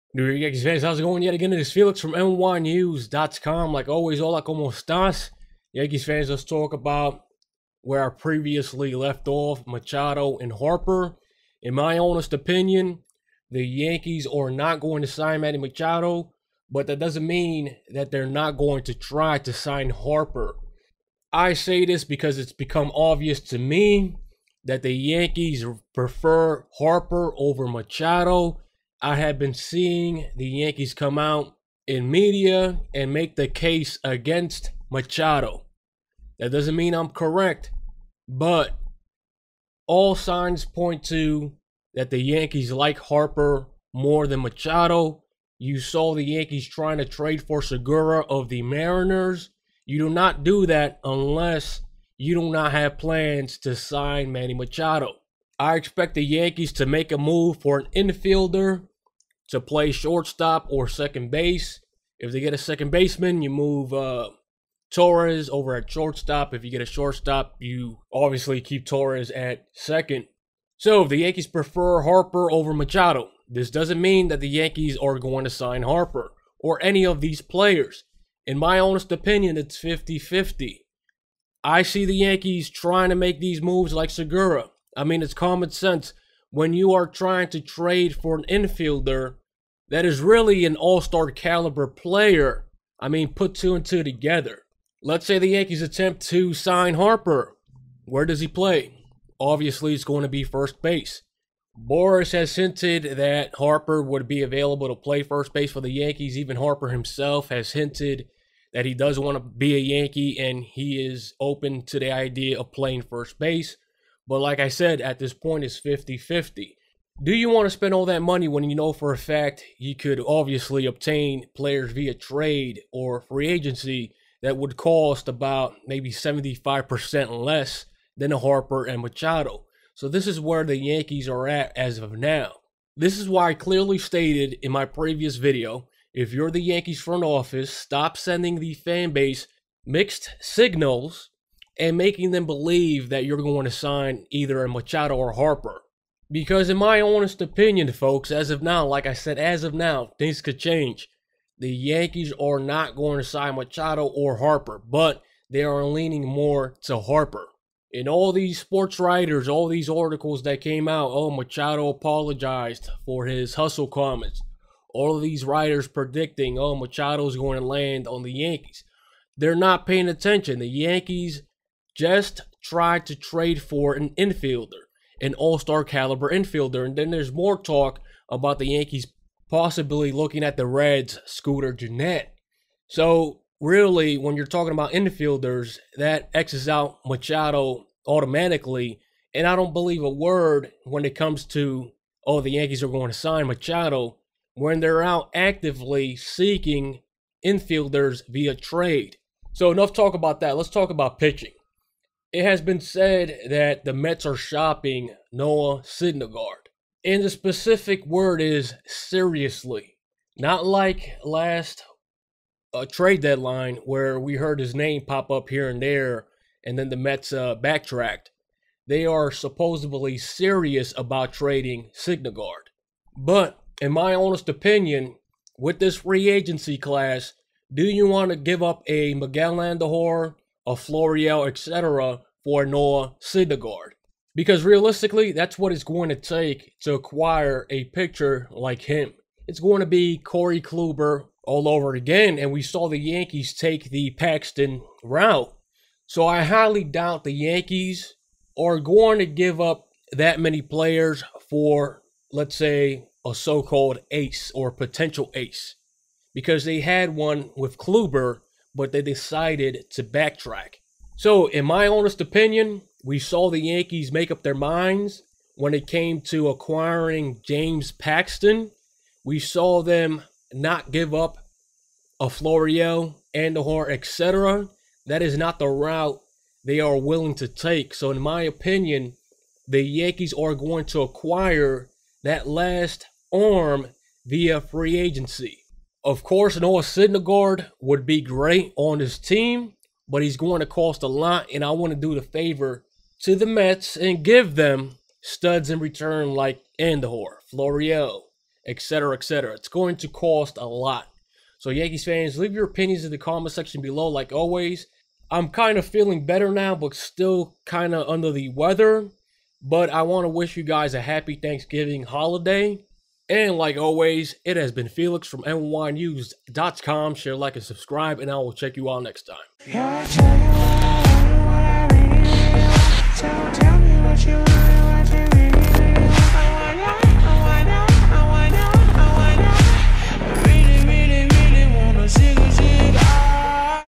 Opinion: Yankees Out On Machado? Leaning More On Harper? + Mets Syndergaard Available
manny machado
bryce harper
mets
yankees
mlb
hot stove
trades